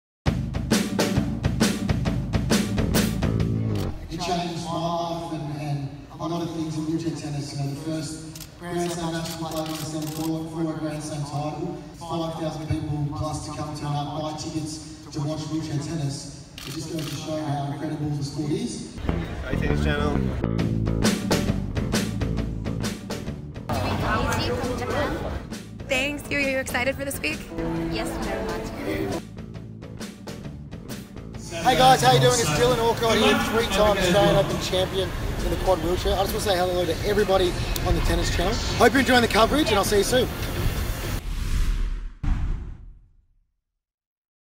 Dylan Alcott, Gustavo Fernandez, Yui Kamiji - Wheelchair Tennis ceremony - Australian Open 2018!!!
Check out these highlights of the Wheelchair Tennis opening ceremony at the Australian Open 2018. All of the champions, and other competitors are there and ready to compete! This is such an awesome event. Absolutely incredible the athleticism that these individuals posses. Love this!